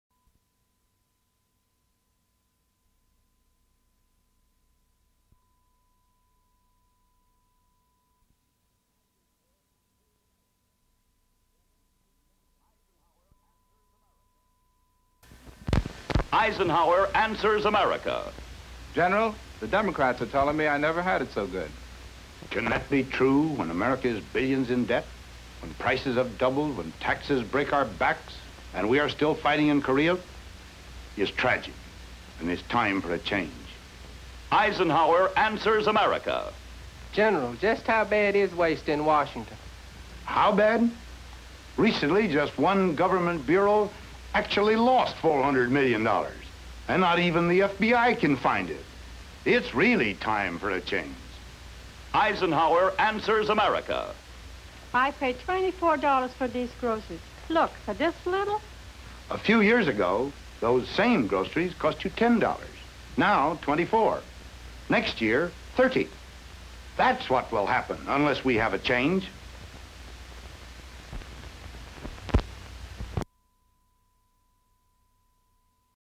Dwight D. Eisenhower [Republican] 1952 Campaign Ad "Eisenhower Answers America"
Eisenhower answers questions about Democrats, waste in Washington, cost of groceries.

Creator: Unknown

Location: United States of America

Format: 16mm Film

Extent: 1 minute 15 seconds

Subjects: Democratic Party, Waste, Economy